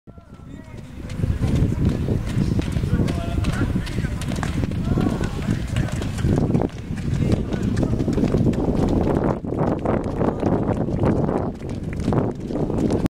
Stage Castres Athlétisme 2018 - J4 - MI et plus - Echauffement 11
Vidéo du stage du Castres Athlétisme 2018 - Journée 4 - MI confirmés et plus

Vidéo prise par Florian Bouleau